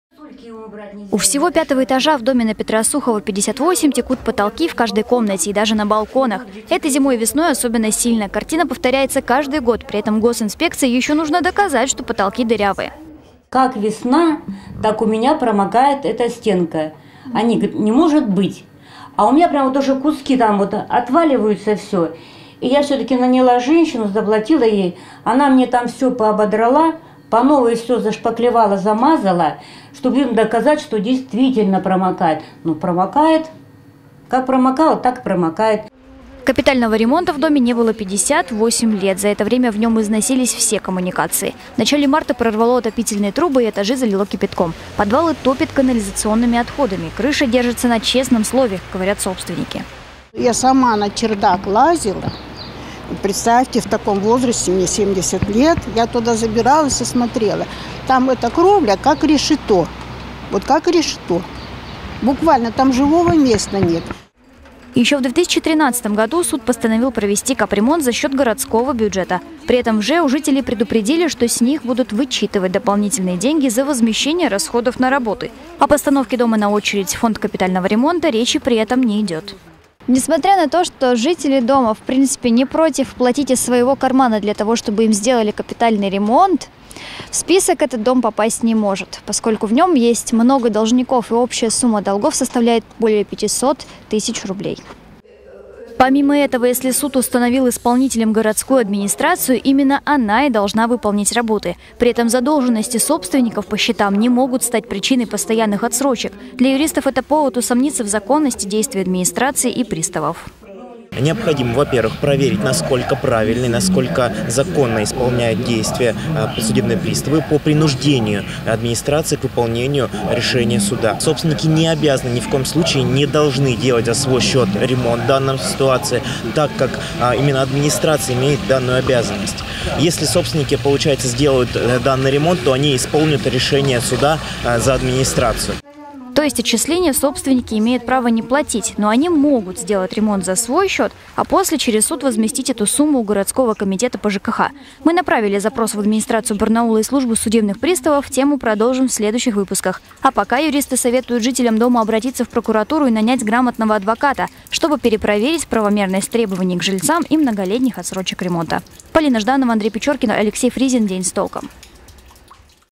60 лет разрушений: барнаульцы даже через суд не могут добиться ремонта дома
Дом по улице Петра Сухова, 58 – почти 60 лет стоит без капитального ремонта. Жители дома больше 7 лет судятся с администрацией города, чтобы им починили крышу и коммуникации. Несмотря на выигранные суды, ремонта нет. Почему и есть ли выход?